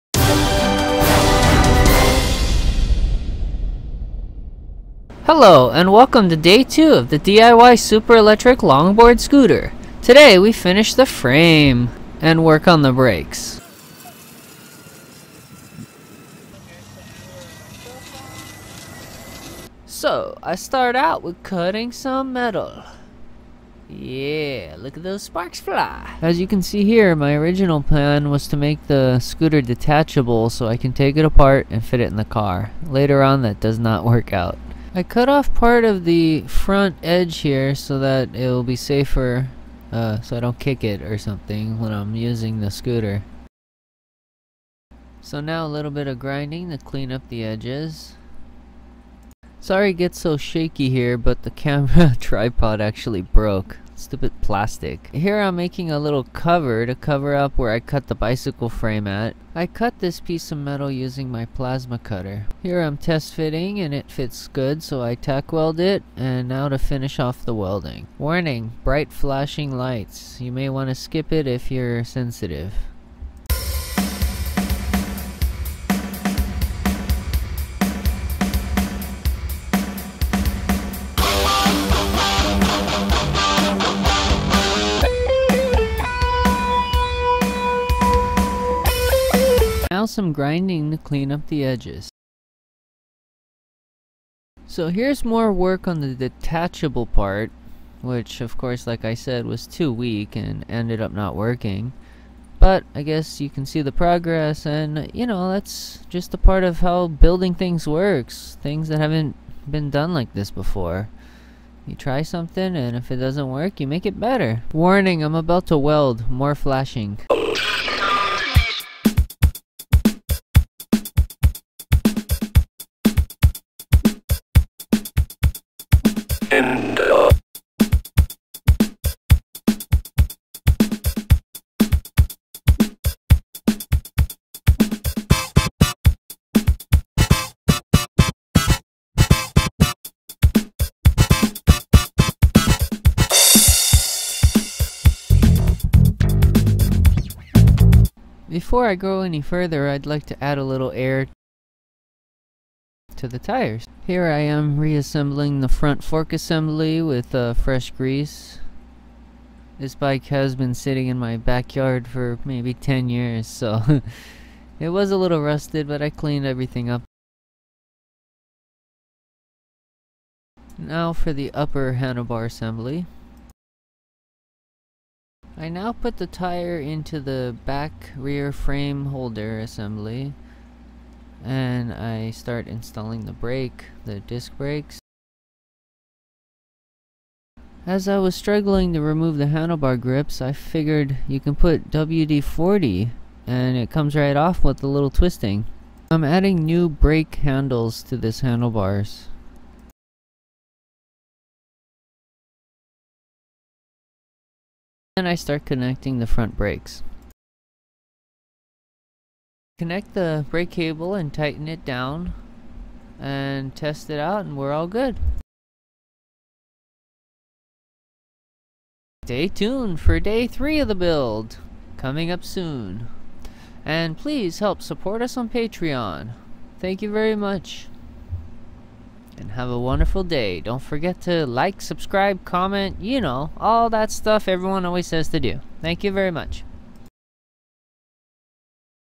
DIY electric scooter build - Day 2 - 1-2-2017 - Frame made - Brakes started
Welcome to Day 2 of the Super DIY electric scooter build 1-2-2017 - Today we finish the Frame Then work on getting the Brakes started.

Questions?

Video Credits:
Edited by: Gabe
Music Composed by: Gabe